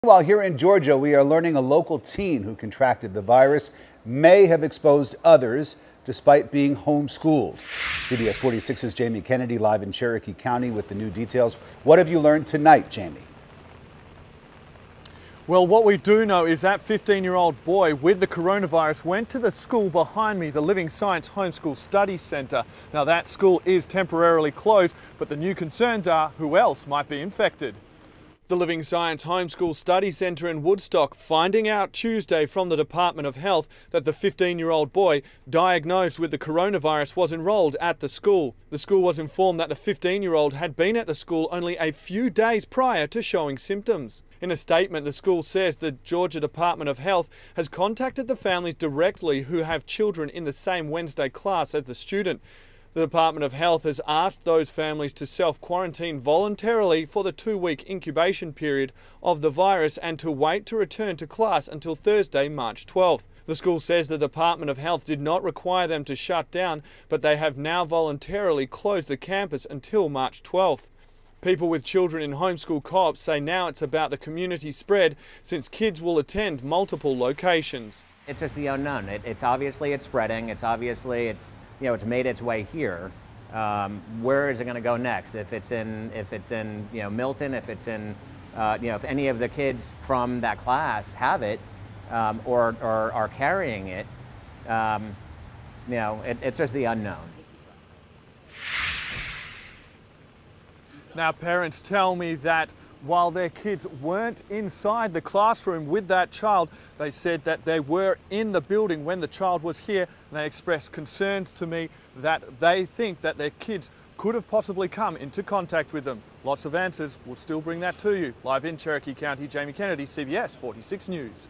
Local school closes for self quarantine for coronavirus
A local school finds out from the department of health that the 15-year-old boy diagnosed with the coronavirus was enrolled at the school.